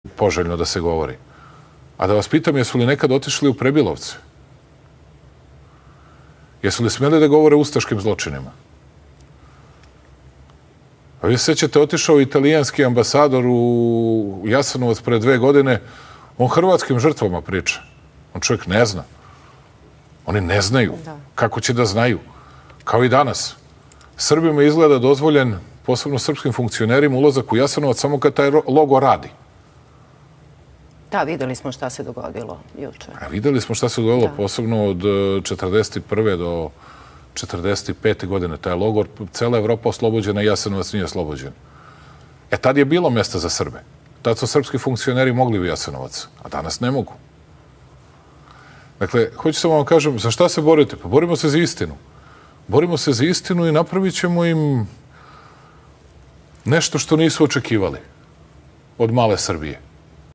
Vucic-A da vas pitam jesu li nekad otišli u prebilovce?Jesu li sme da govori o ustaškim zločinima?
Poželjno da se govori.
A da vas pitam jesu li nekad otišli u prebilovci?Jesu li sme da govori o ustaškim zločinima?Sve ćete otišao i talijanski ambasador u jasan od pre 2 godine u hrvatskim žrtvama priče čovjek ne zna.
Oni ne znaju kako će da znaju, kao i danas, srbima izgleda dozvoljen posebno srpskim funkcionerima.
Ulazak u jasenovac logor radi.
Ta, vidjeli smo šta se dogodilo jučer.
Hod 40 i 1 do 45 godine.
Taj logor cela evropa oslobođena jasenovac nije oslobođen.
E tad je bilo mjesta za srbe da su srpski funkcioneri mogli u jasenovac, a danas ne mogu.
Dakle, hoću da vam kažem šta se borite, pa borimo se za istinu.
Borimo se za istinu i napravit ćemo im.
Nešto što nisu očekivali.
Od male srbije.